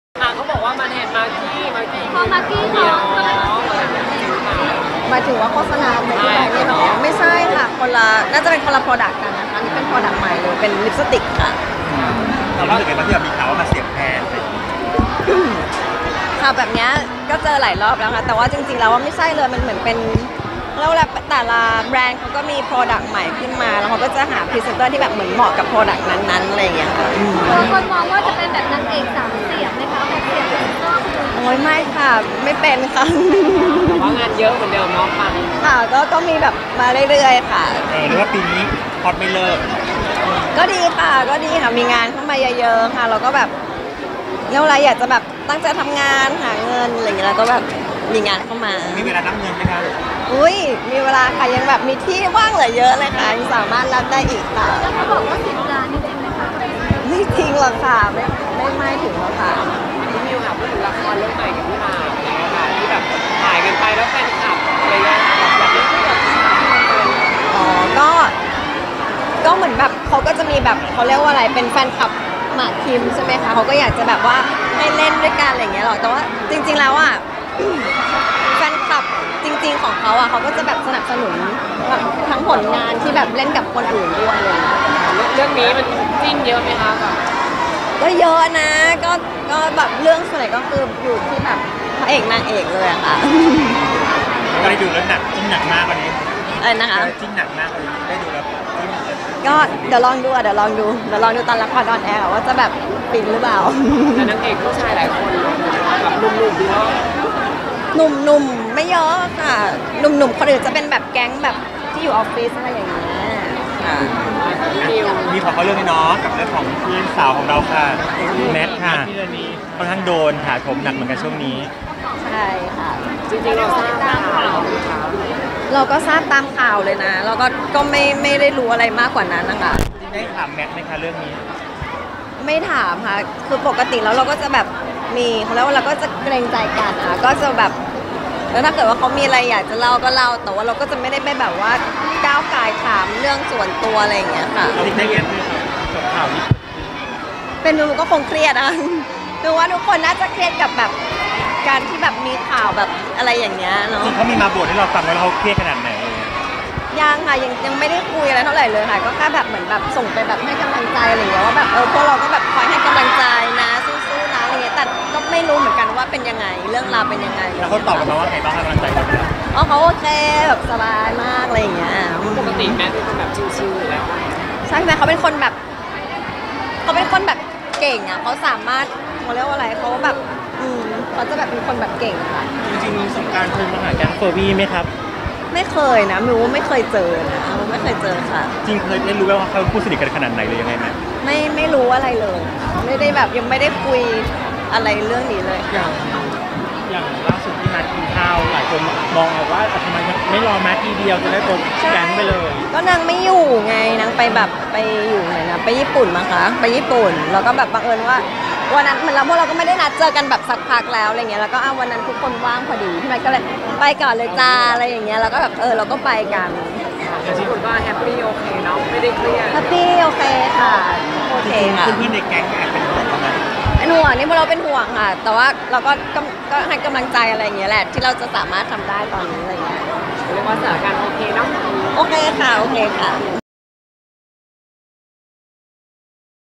มิว นิษฐา จะตอบแทน แมท ยังไง หลังโดนจับผิดเที่ยวบาร์ลับ-ความสัมพันธ์ กับ สงกรานต์
มิว นิษฐา ปัดเสียบงานนี้แทน มาร์กี้ ราศรี เพราะท้อง แจงเป็นคนละตัวกัน รับมีงานเข้ามาเรื่อยๆ ลั่นค่าตัวไม่ถึง10ล้าน เปรยรู้ข่าว แมท ภีรณีย์ ตามข่าวเลย ตนไม่รู้อะไรมากกว่านั้น ลั่นไม่ถามเพราะมีความเกรงใจกัน ไม่ก้าวก่ายเรื่องส่วนตัวกัน เชื่อเป็นใครเจอข่าวแบบนี้ก็คงเครียด ตนและเพื่อนในแก๊งก็เป็นห่วงมีให้กำลังใจกันบ้าง ยันส่วนตัวไม่เคยเจอสงกรานต์ แจงล่าสุดที่นัดกินข้าวกันไม่มีแมทเพราะอีกฝ่ายไปญี่ปุ่น

แม่น!!!กว่า10000 คนเช็คดวง12ราศีและเสริมดวงรวยกับCute Club โทร*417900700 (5 บาทต่อsms/ 2 sms ต่อวัน)